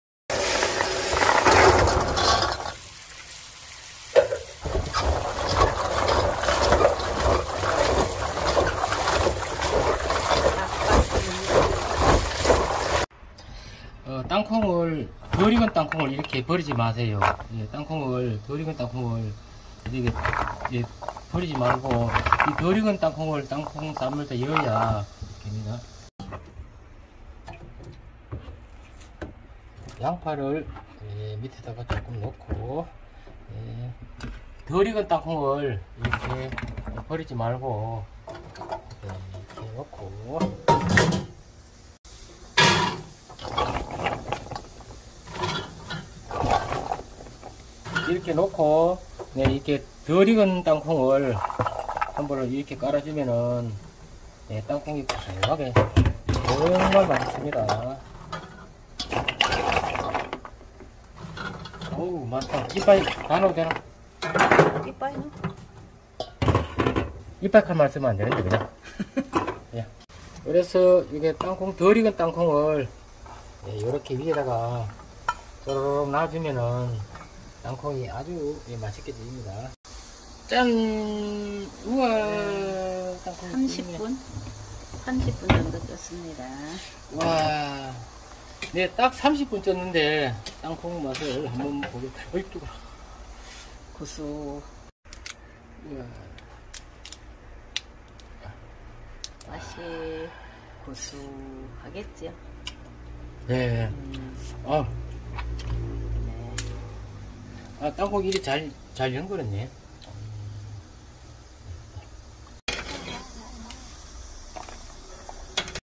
땅콩🥜(햇땅콩)이것꼭! 넣고 찌세요 쿨 땅콩맛
# 햇땅콩 바로깨서 삶을때 설익은 어린땅콩을 버리지말고 함께 깔아서 찌면 정말 구수하고 맛이월등하게 좋아요